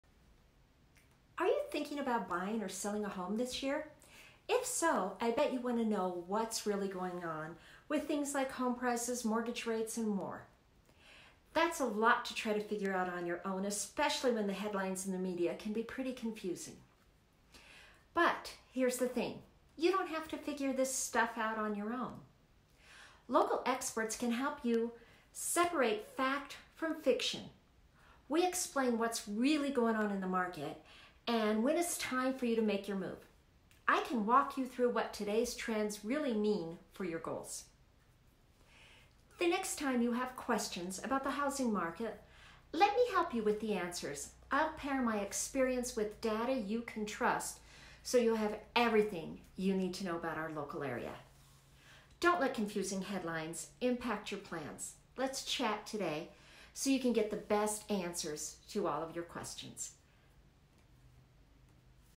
2/17/23 | REalTalk with Debi | Fact or Fiction?
What's really going on with the real estate market?

Answering the Questions I am often asked.